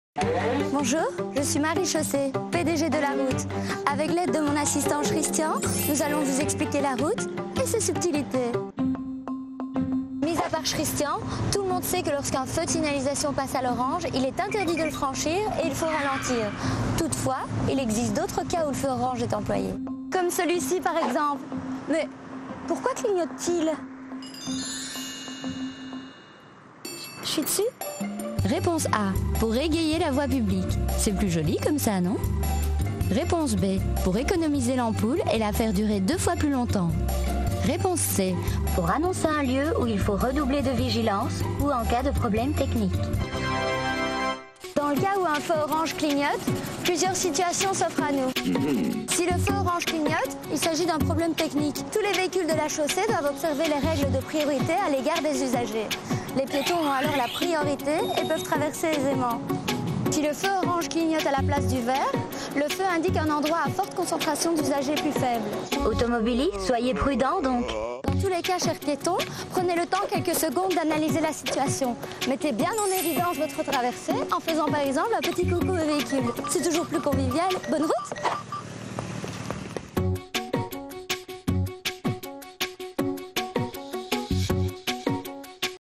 Feu orange
Avec Marie-Chaussée, pdg de la route, découvrez avec humour les subtilités de la sécurité routière à Bruxelles.